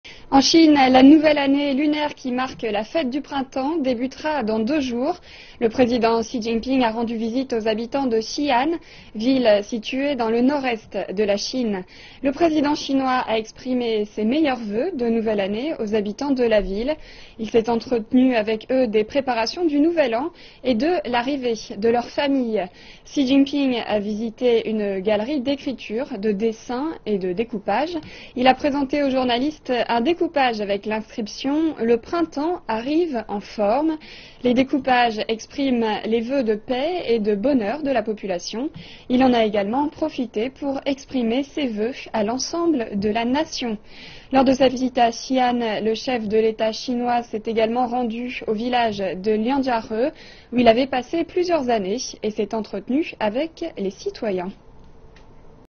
Xi Jinping à Xi'an pour présenter ses vœux pour la nouvelle année
A deux jours du début de la Fête du Printemps, le Président chinois Xi Jinping s'est rendu à Xi'an pour présenter ses vœux à la population.